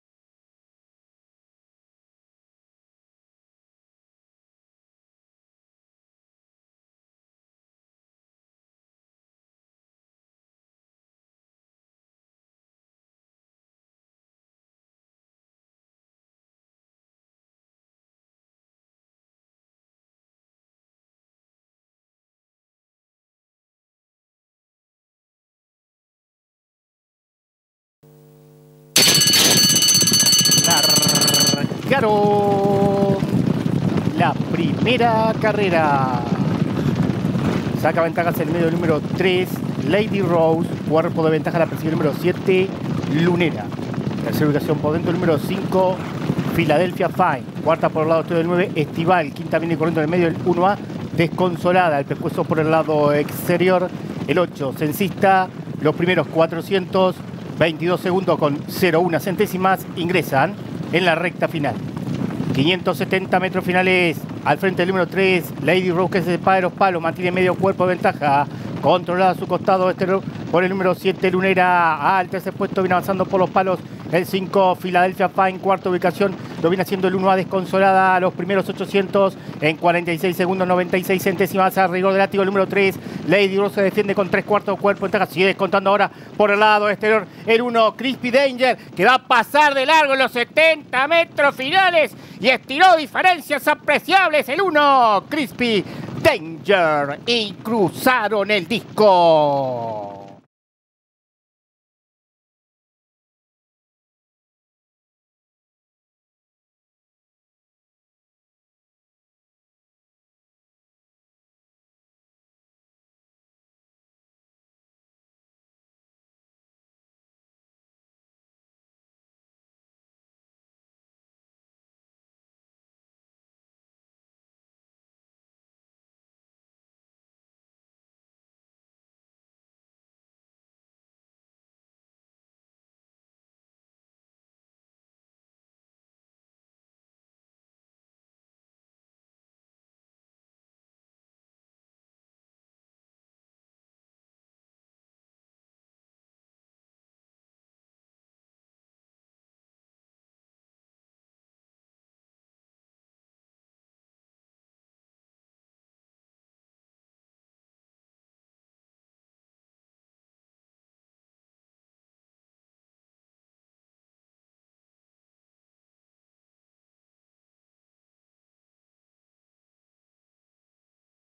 18/01/2015 – Hipodromo Maroñas – Carrera 1 – CUCUEL (2012)
Carrera 1 realizada el dia 18/01/2015 por el premio CUCUEL (2012) en Hipodromo Maroñas